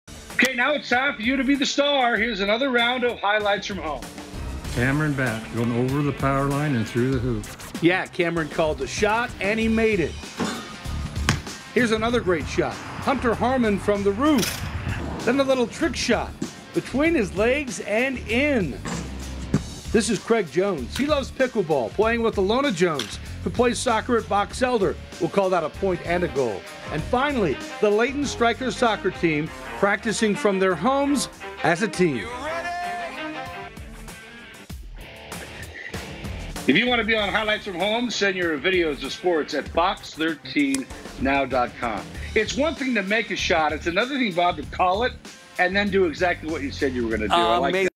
Highlights from Home: Monday 5/11 (Part Two)
Crazy basketball trick shots and creative soccer practice methods are featured!